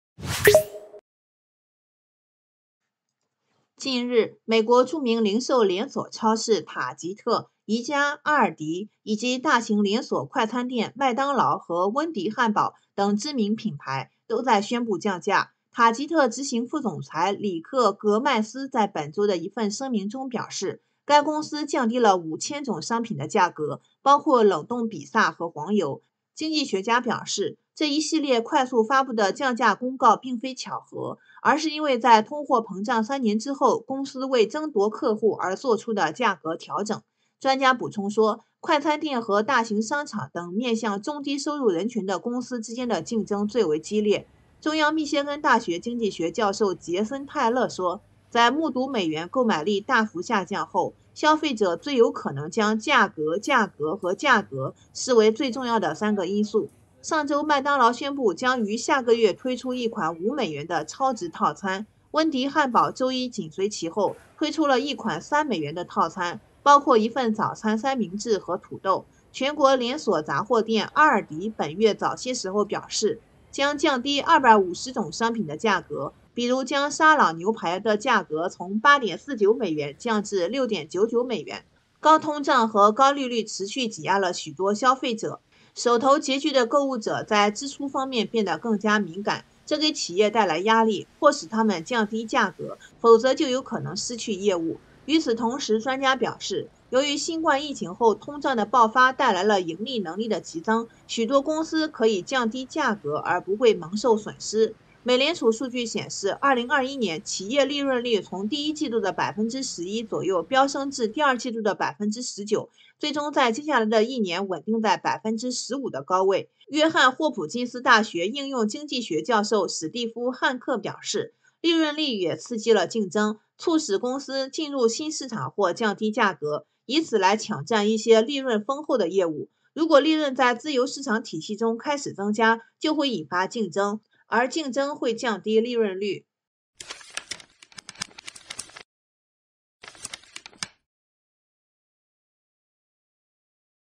美国快餐连锁、零售超市宣布降价，价格战爆发了？
近日，美国著名零售连锁超市塔吉特（Target）、宜家、阿尔迪（Aldi），以及大型连锁快餐店麦当劳和温迪汉堡等知名品牌，都在宣布降价。